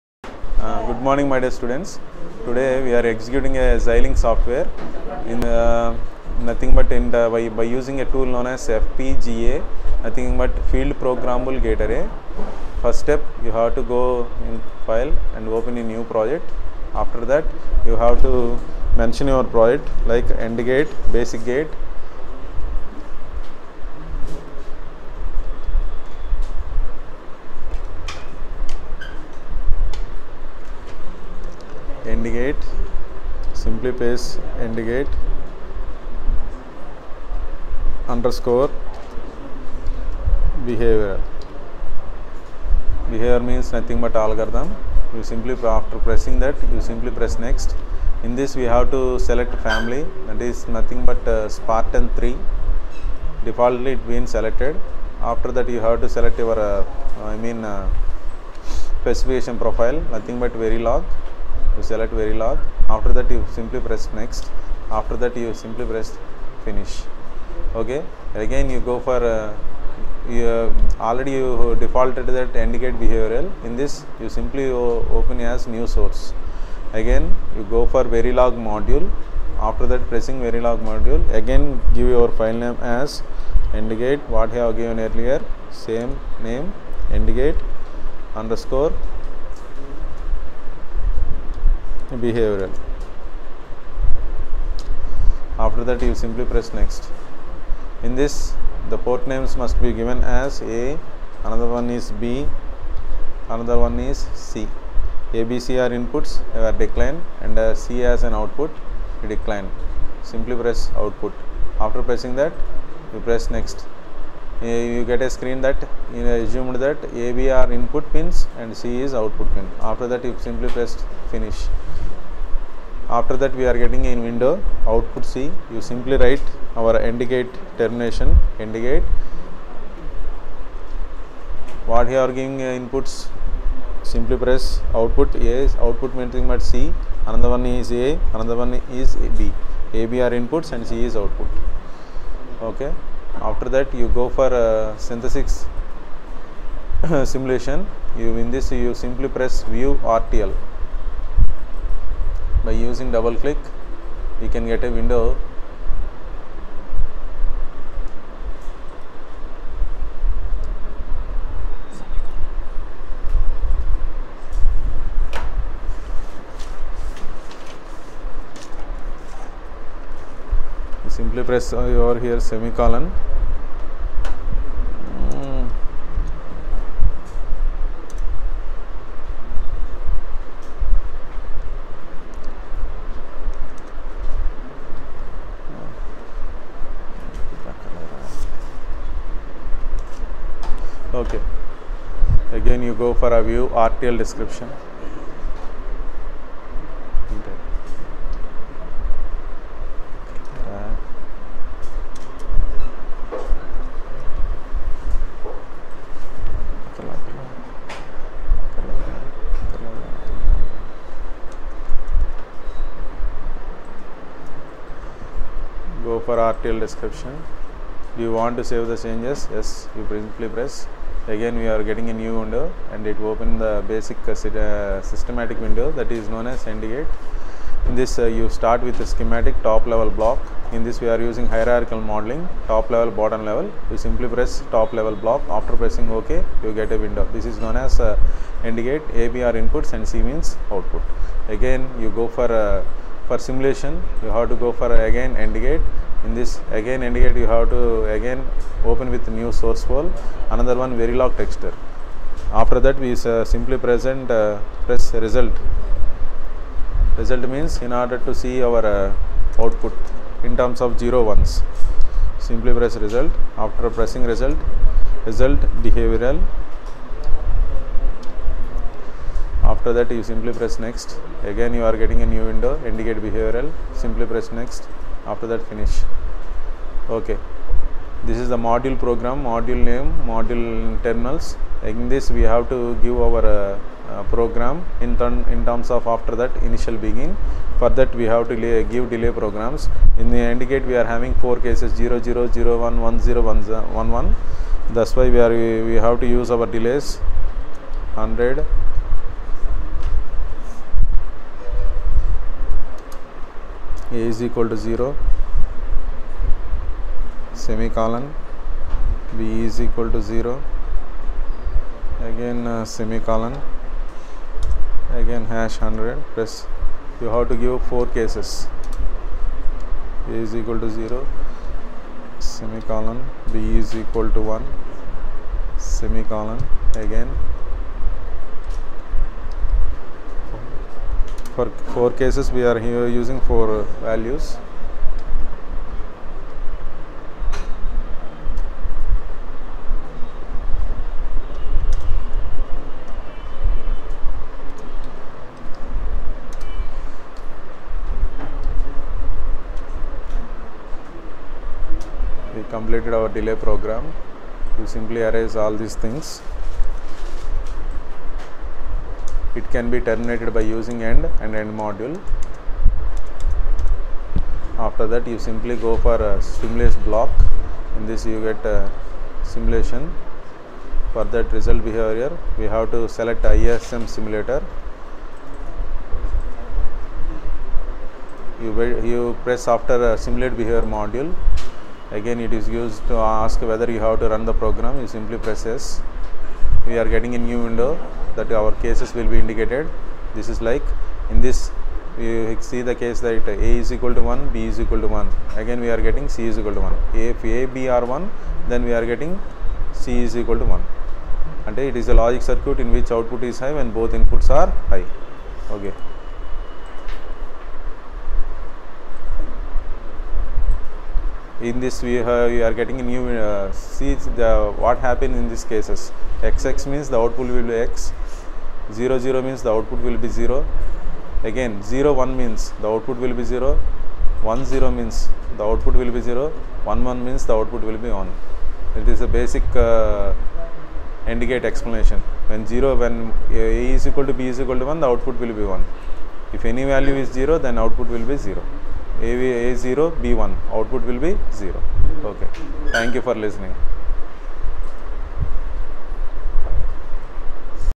Xilinx Verilog 14.2 Version Execution
Hi Everyone,
I K.S.V.Samba Shiva Head Of  Department - Electronics , Parvathaneni Brahmayya Siddhartha College of Arts & Science, Siddhartha Nagar, Vijayawada-520010, Andhra Pradesh, India
This Video is about How to execute Xilinx Verilog 14.2 version